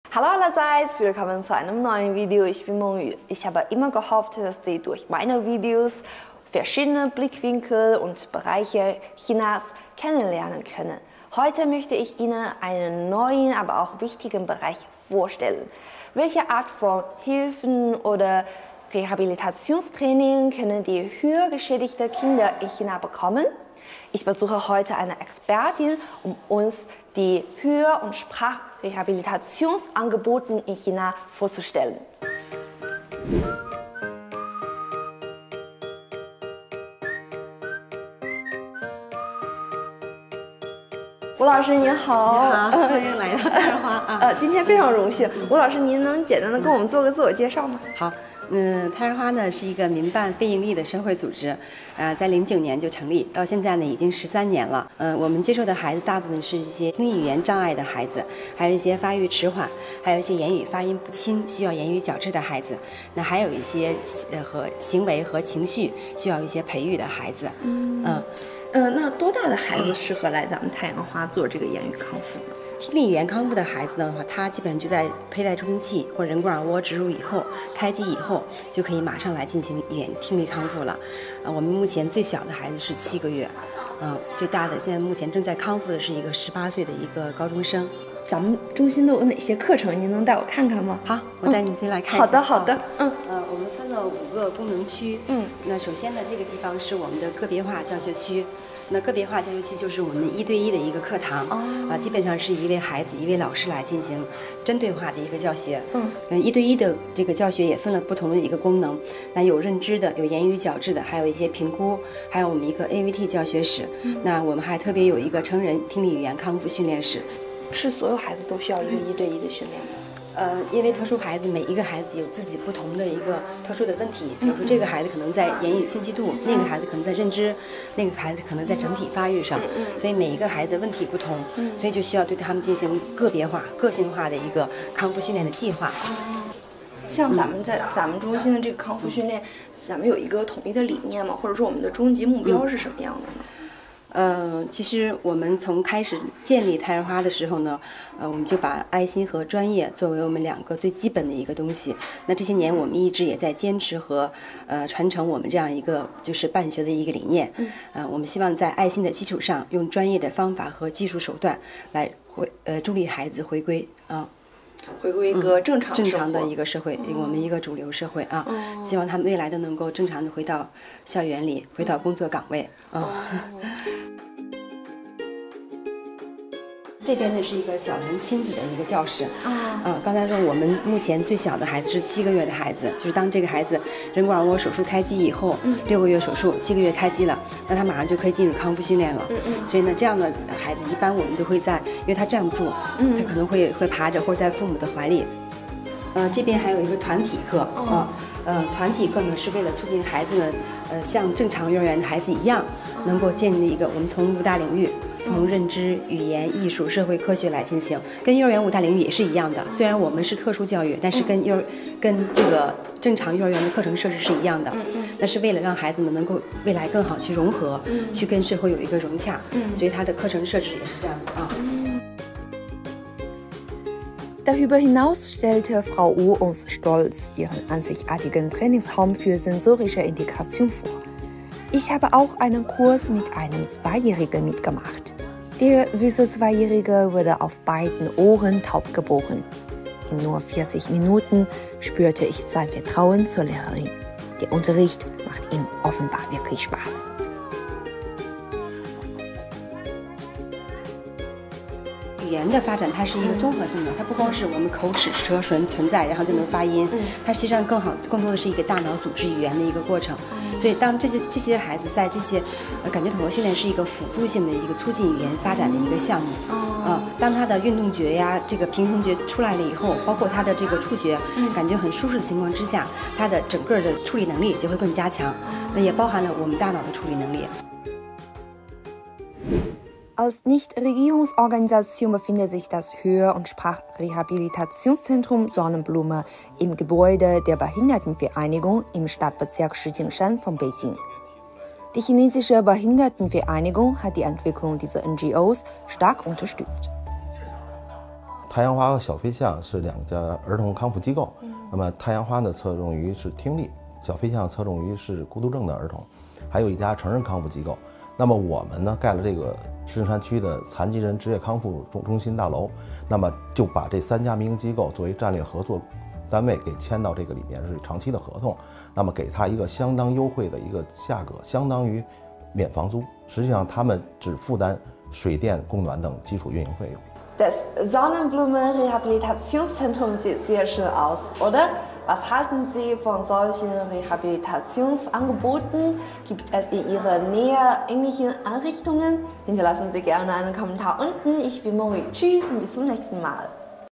Wie sieht ein Hör- und Sprachrehabilitationszentrum in China aus?
Ich habe immer gehofft, dass Sie durch meine Videos verschiedene Blickwinkel und Bereiche Chinas kennenlernen können. Wir haben bereits gemeinsam ein Altenheim und eine Universität in China besucht. Und heute möchte ich Ihnen einen neuen, aber auch sehr wichtigen Bereich vorstellen. Welche Art von Hilfe und Rehabilitationstraining können hörgeschädigte Kinder in China bekommen? Ich habe heute eine Expertin eingeladen, um uns die Hör- und Sprachrehabilitation in China vorzustellen.